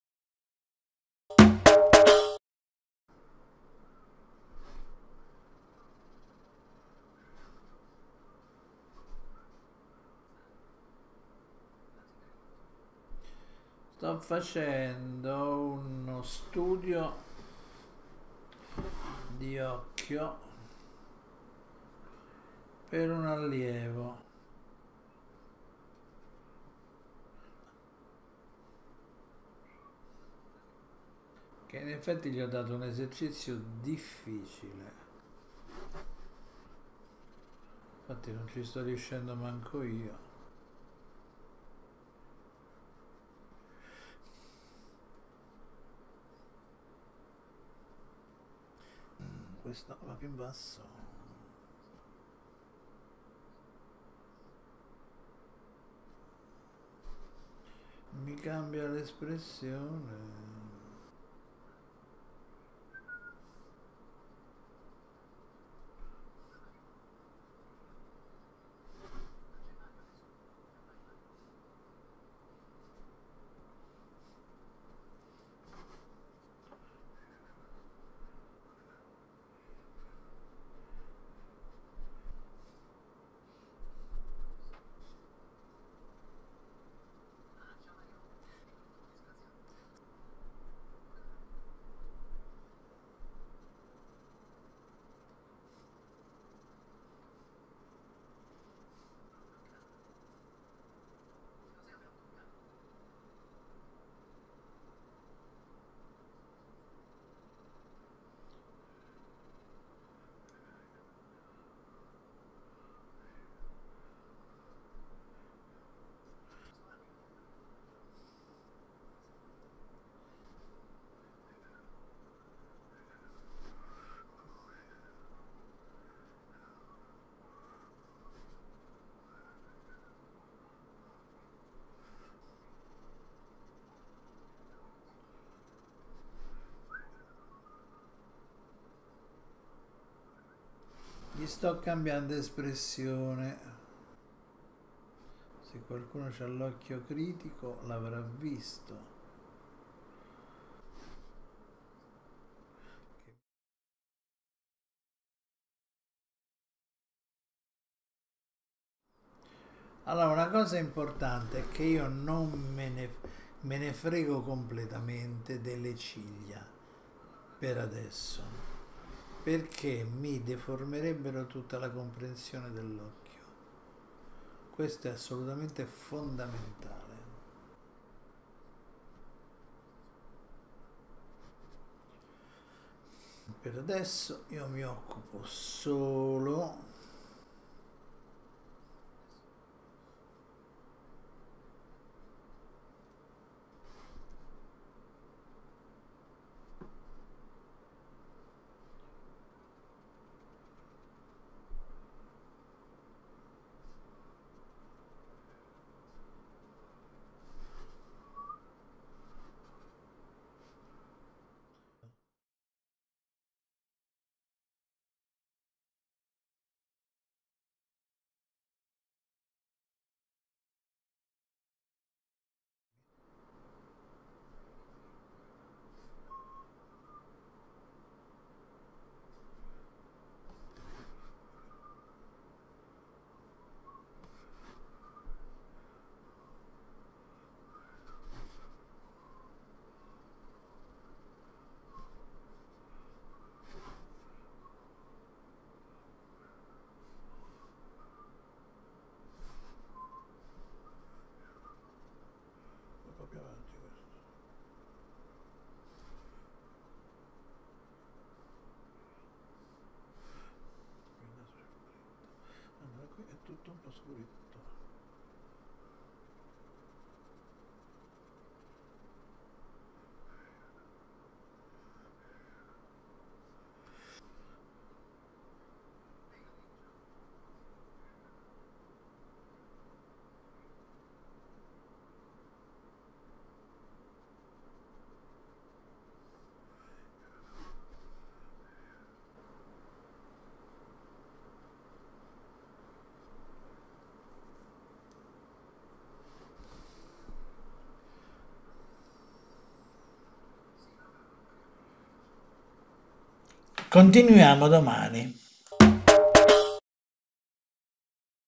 Uno studio di occhi (4)
Se procedi senza aggrapparti alle linee (che non esistono) e continui a modellare il chiaroscuro piano piano la forma che stai cercndo di definire si materializza in modo fluido e tu puoi continuare a correggere il tiro fino alla fine.

* Fai gli esercizi e ricevi riscontro nel forum dedicato